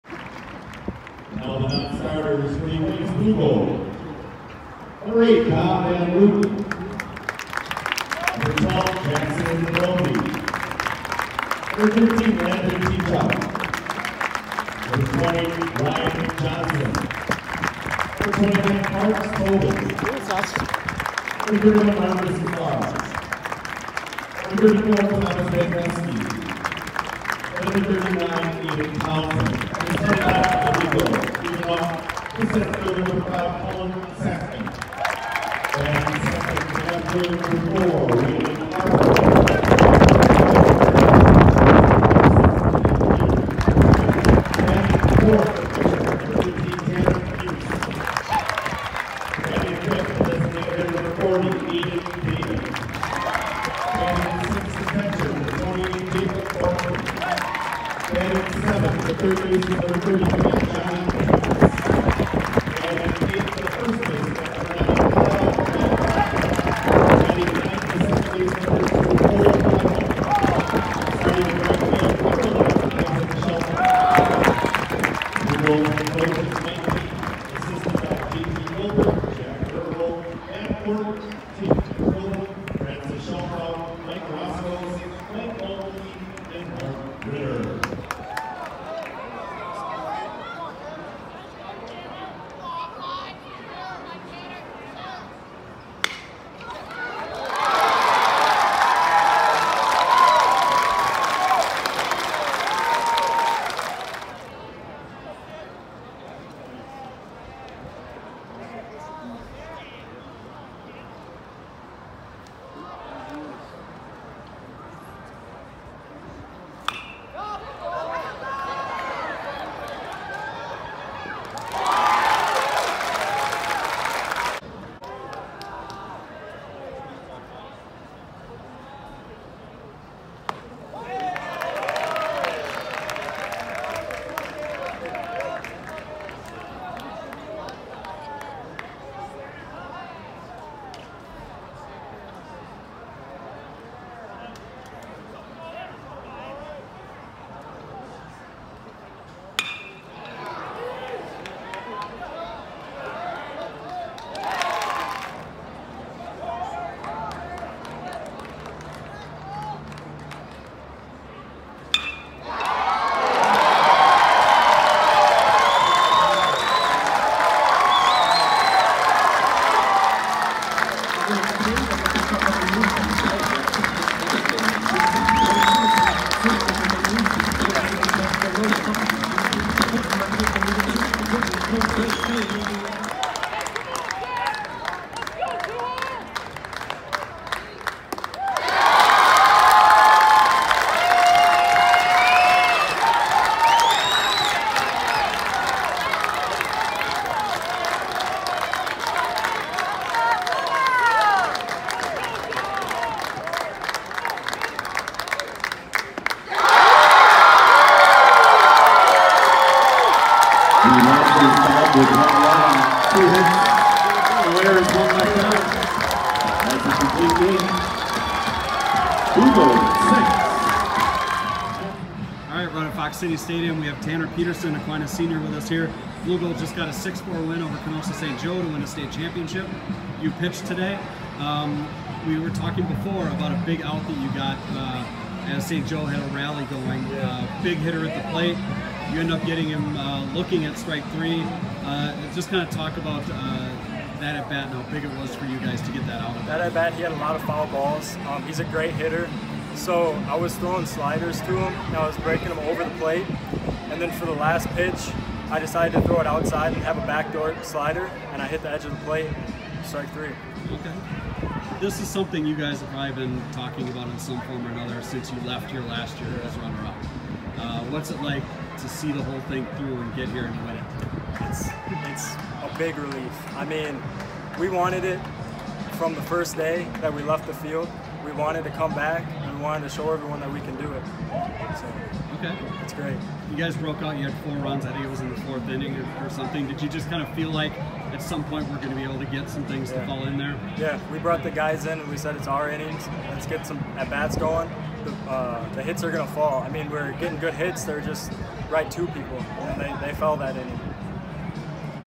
WIAA D3 championship baseball game: La Crosse Aquinas 6, Kenosha St. Joseph 4
The Aquinas High School baseball team beat Kenosha St. Joseph 6-4 in the WIAA Division 3 championship game.